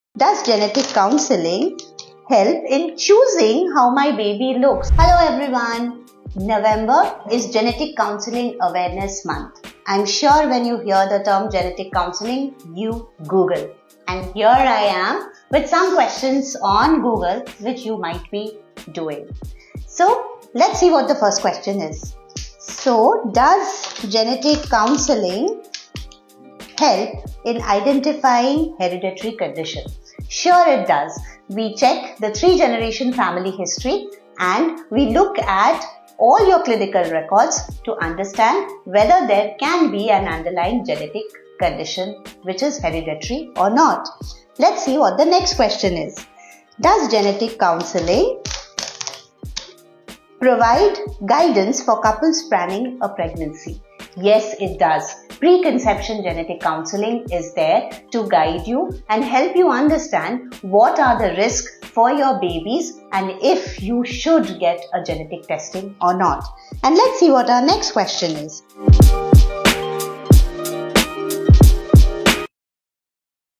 Most Googled Questions on Genetic Counselling (Part-1)
It’s Genetic Counselling Awareness Month—a time to explore the powerful impact of understanding our genetics. 🧬✨

Join us as Dr. Dipanjana Dutta, our genetic counsellor at Renew Healthcare, addresses some of the most Googled questions about genetic counselling, helping to demystify the process.

What questions do you have about genetic counselling? Share them in the comments below! 💭